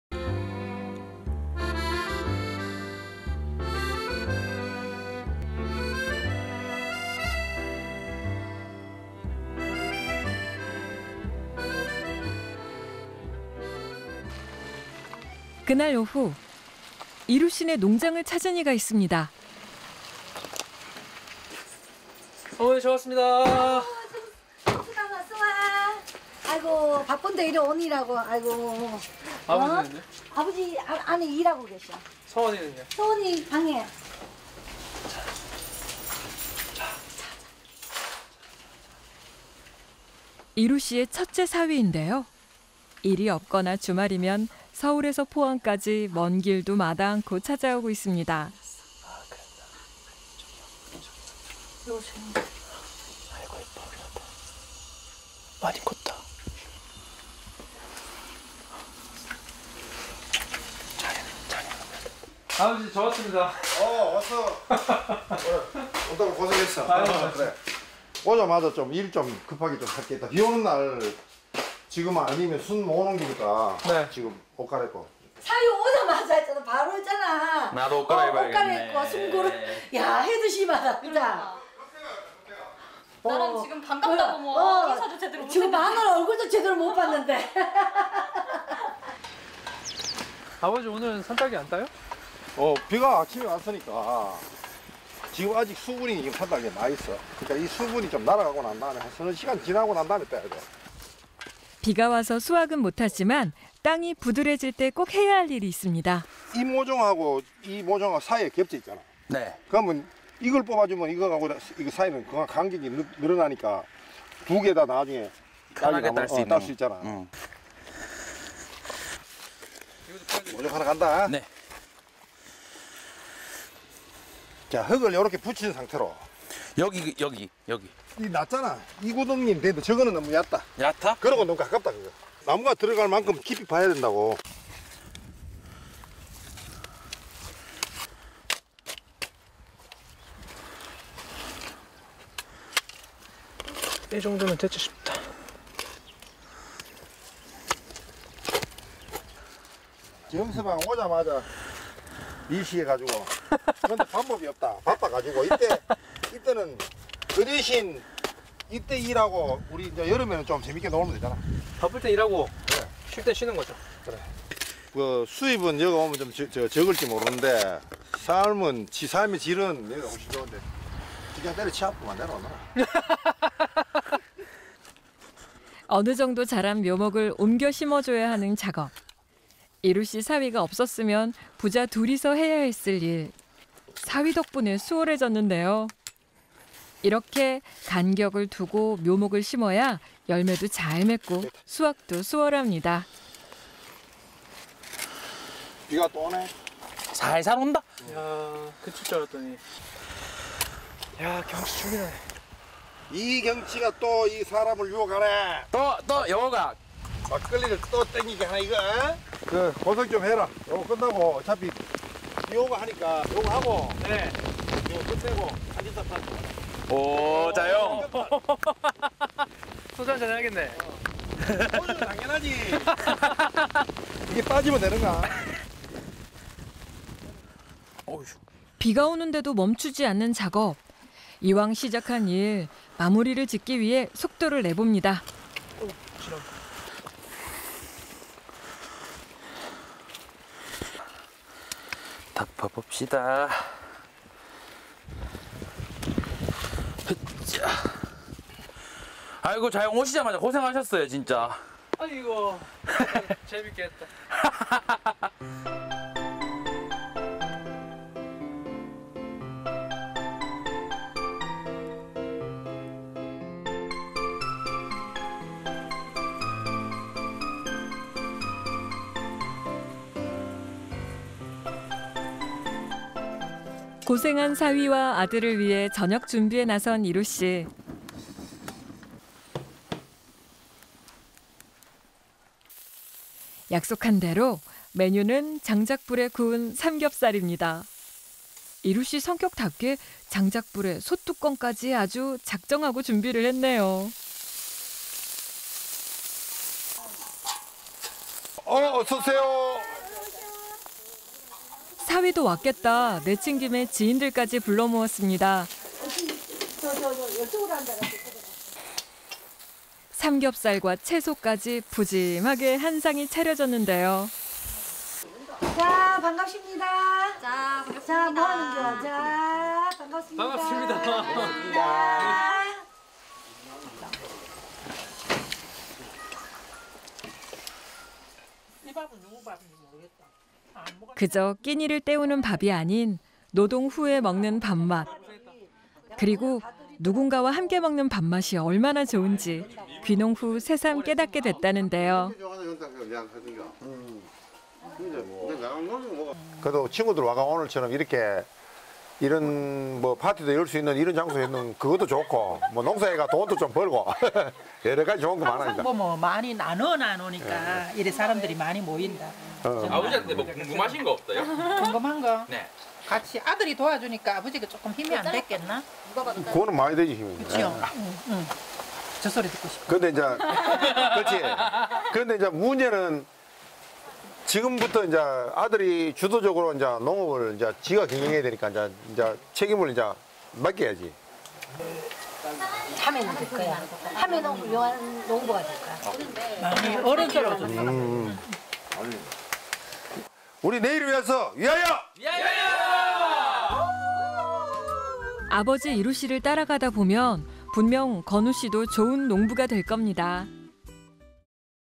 [산딸기 부자의 농사 정복기] 새삼 깨달은 귀농의 즐거움
고생한 사위와 아들을 위해 저녁 준비에 나선 일우 씨
약속한 대로 메뉴는 장작불에 구운 삼겹살입니다

일우 씨 성격답게 장작불에 솥뚜껑까지
아주 작정하고 준비를 했네요

사위도 왔겠다 내친김에 지인들까지 불러 모았습니다
삼겹살과 채소까지 푸짐하게 한 상이 차려졌는데요

그저 끼니를 때우는 밥이 아닌
노동 후에 먹는 밥맛

그리고 누군가와 함께 먹는 밥맛이 얼마나 좋은지
귀농 후 새삼 깨닫게 됐다는데요